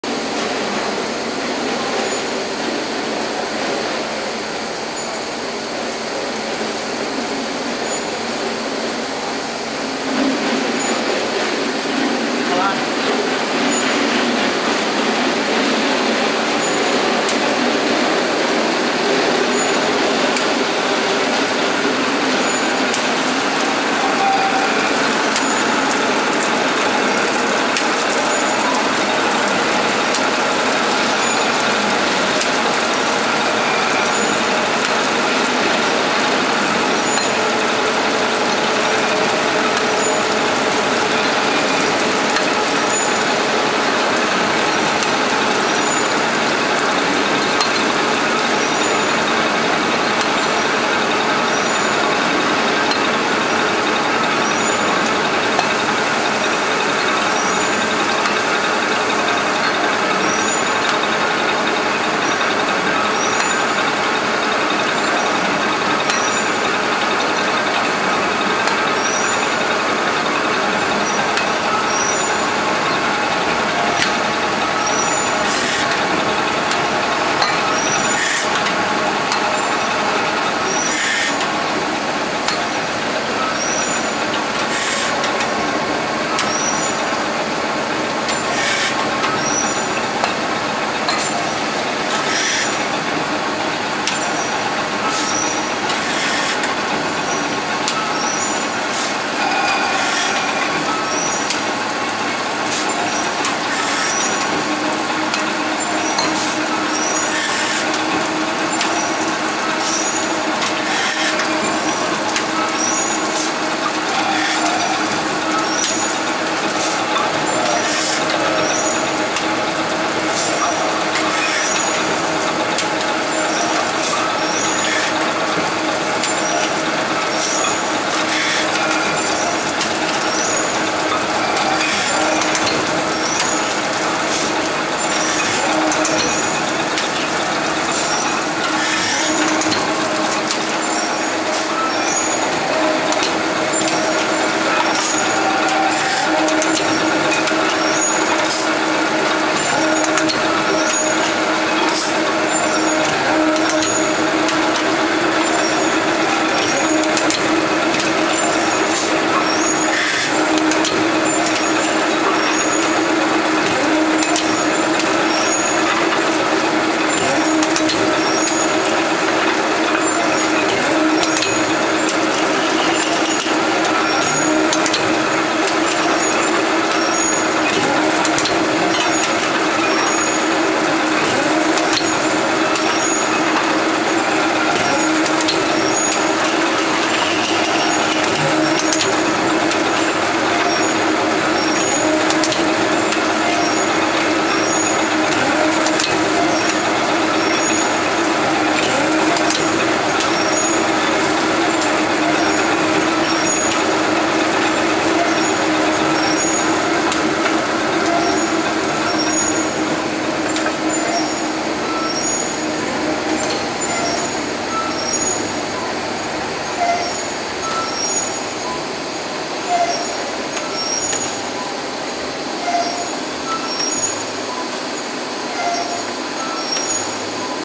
RGF-12-12-1 Glass bottle aluminum cap beer filling and capping machine
Washing heads: 12pcs
Filling heads: 12pcs
Capping heads: Single head
Filling volume: 100ml-750ml
Filling speed: 500-1000bph
Filling type: Isobaric filling machine
Filling liquid: Beer, carbonated drinks, etc.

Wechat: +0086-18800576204
Skype: monika.yao1
Factory add: Wukesong road , Sanxing Street, Jinfeng Town, Zhangjiagang city, Jiangsu Province, China 213100